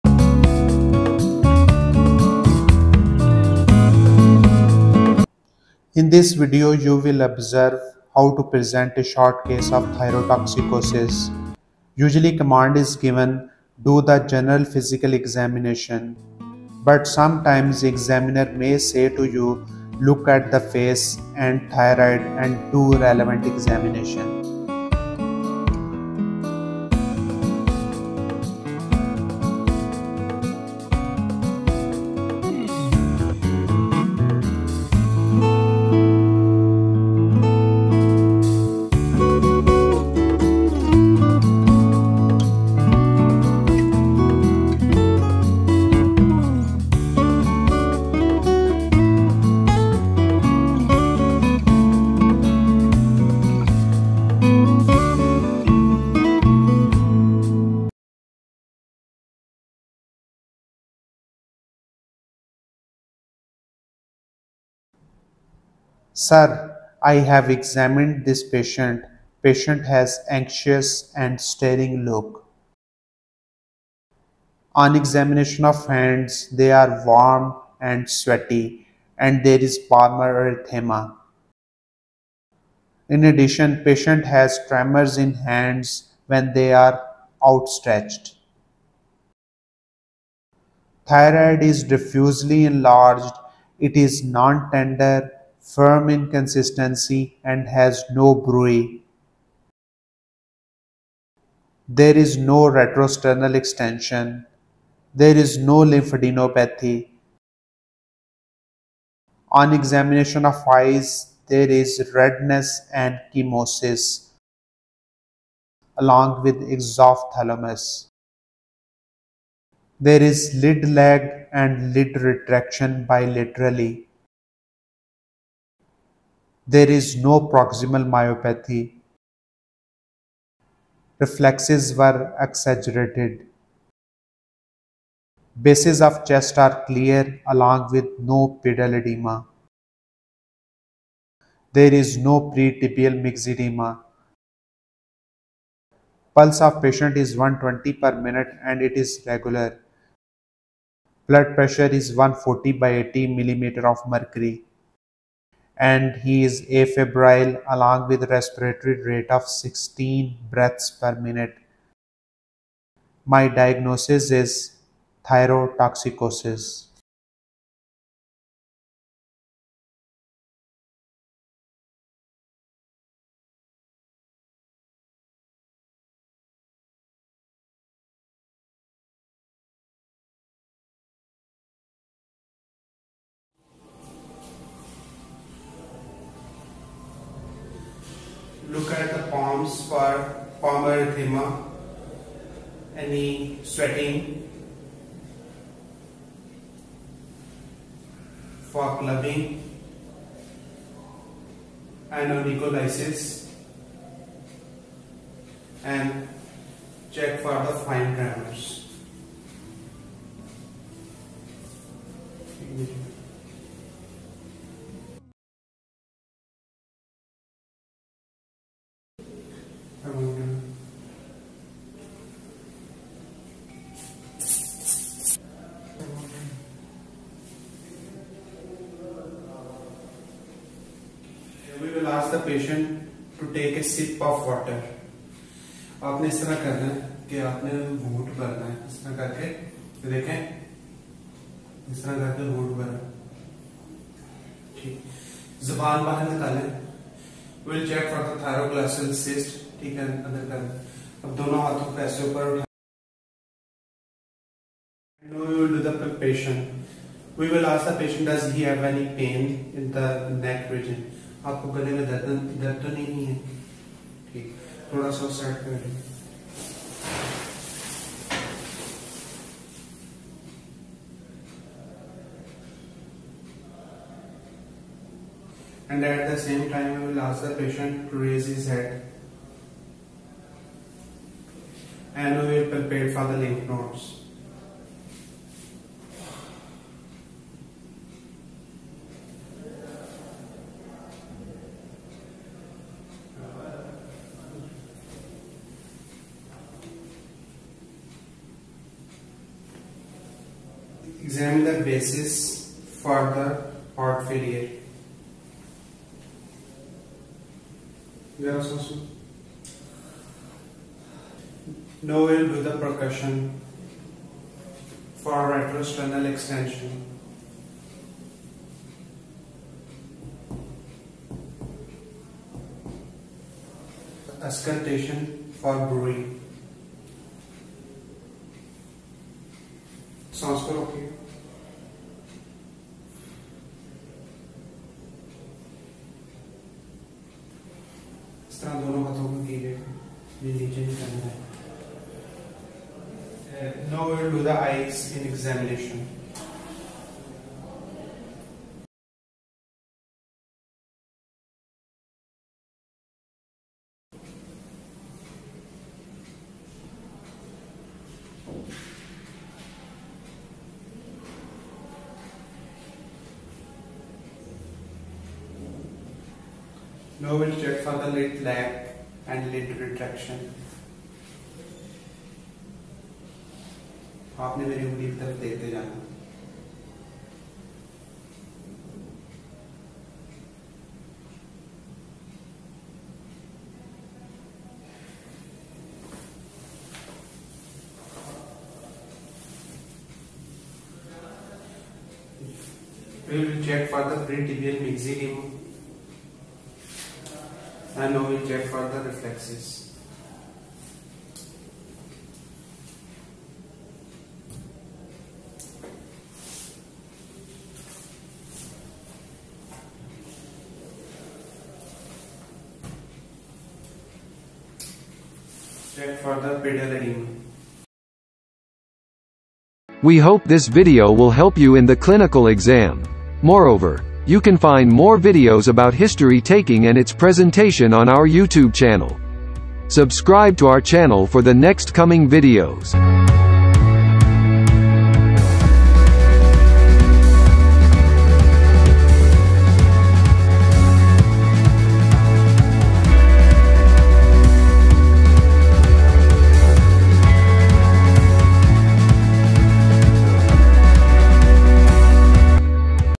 Thyrotoxicosis ( Short Case )
This video is to help medical students to improve their knowledge.
This video is to help medical students. In this video, you will observe how to examine and present a short case of thyrotoxicosis in the exam in an appropriate manner.